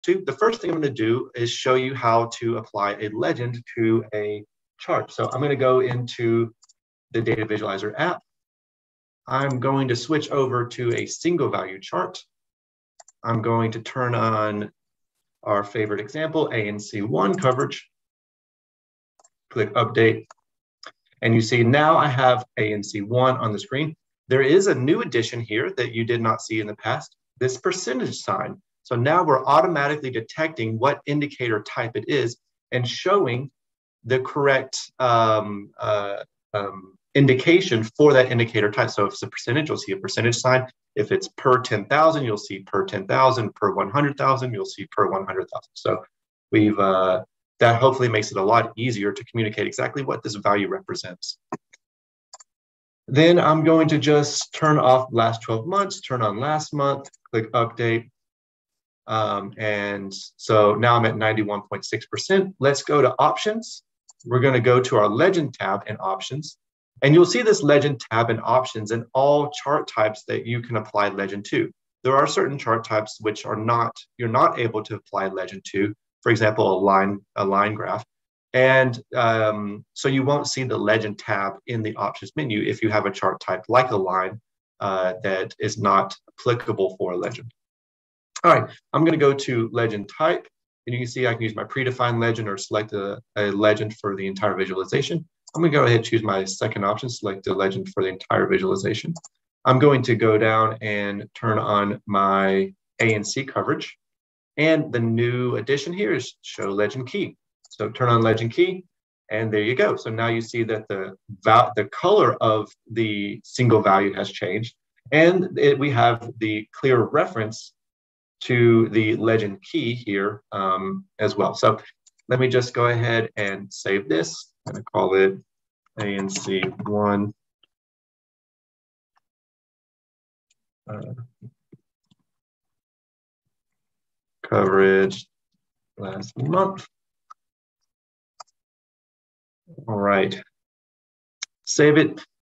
DHIS v2.37 Analytics - Legends in Data Visualizer
Recording from a live demonstration of new features in DHIS2 version v2.37. This video demonstrates new Legends in Data Visualizer features.